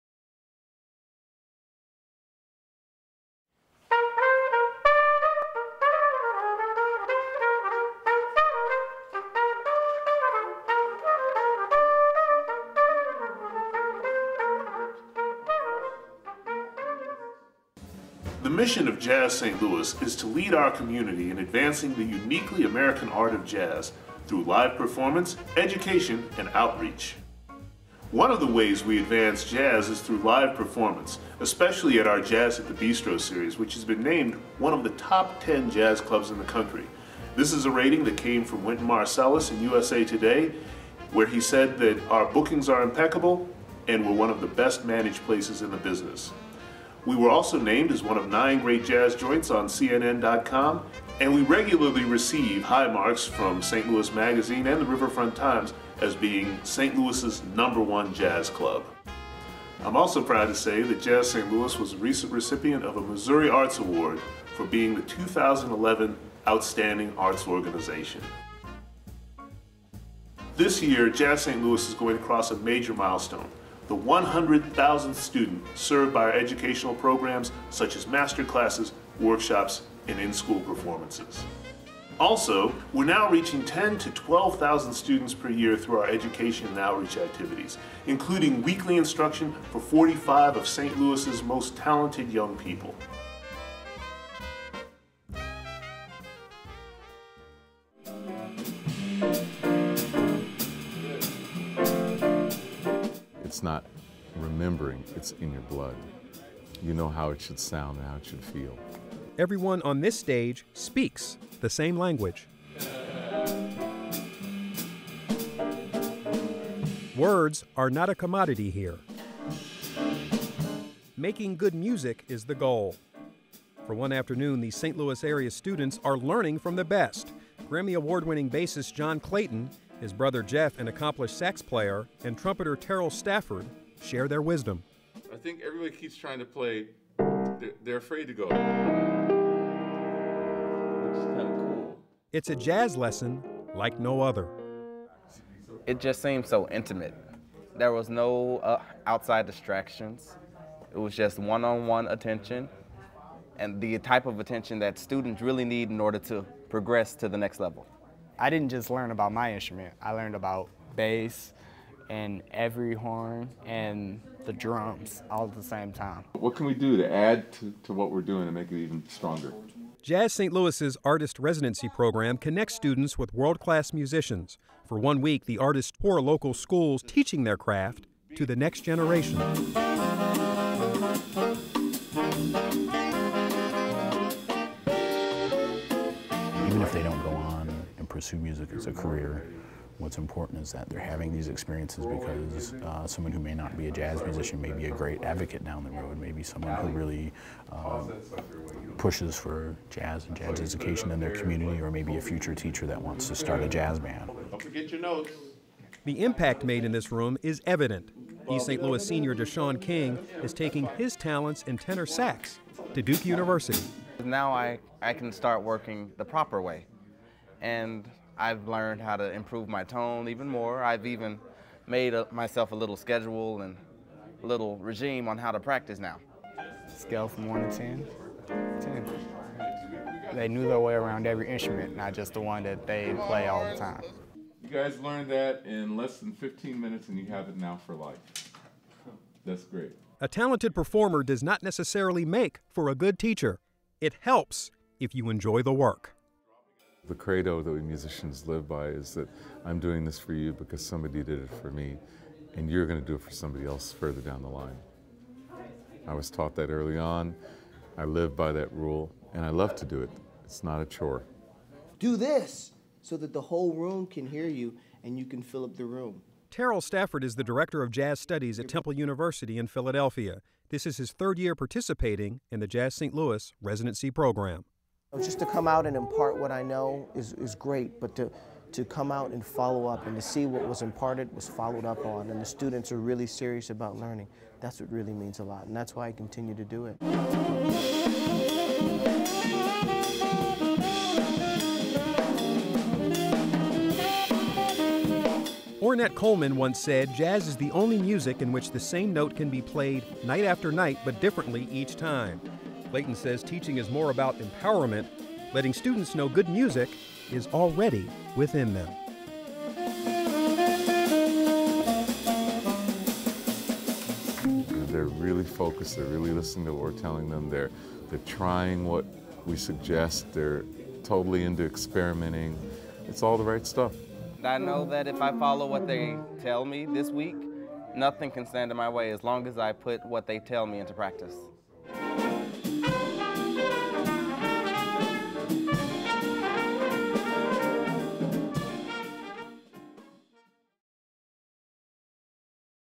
What is Jazz St. Louis? 2011 Edition
Executive Director Gene Dobbs Bradford talks about Jazz St. Louis. This video features a segment by HEC-TV focusing on the JSL Artist Residency program.